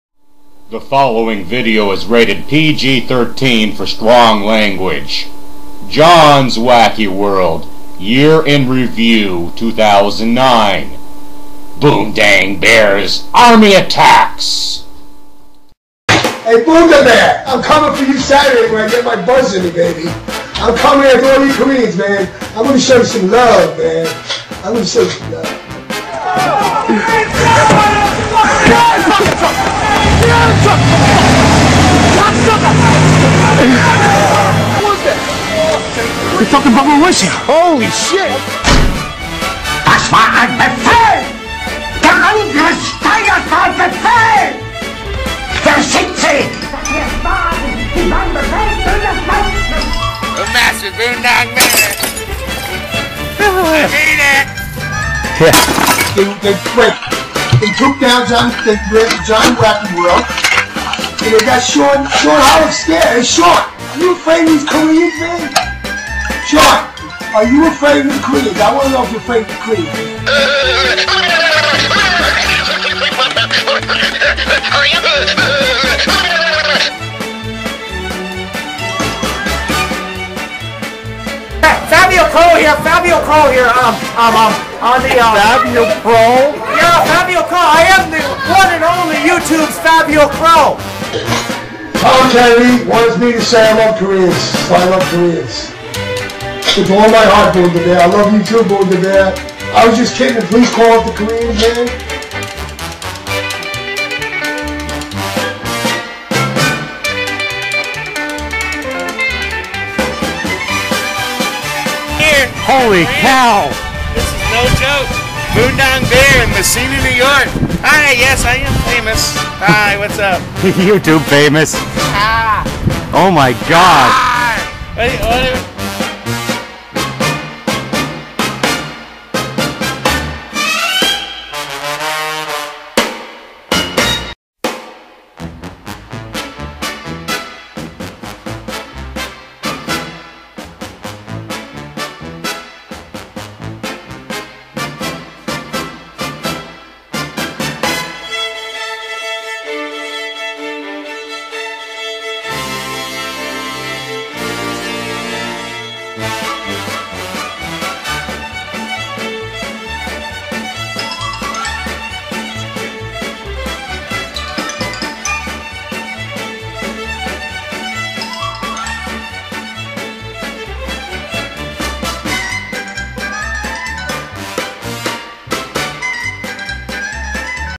Johnswackyworld Year In Review 2009 -- Bundangbear's Army Attacks
this johnswackyworld year in review for 2009 covers the war and outcome of peace between jim riley and bundangbear over cyber bullying on youtube. jan 7, 2010 uploaded to youtube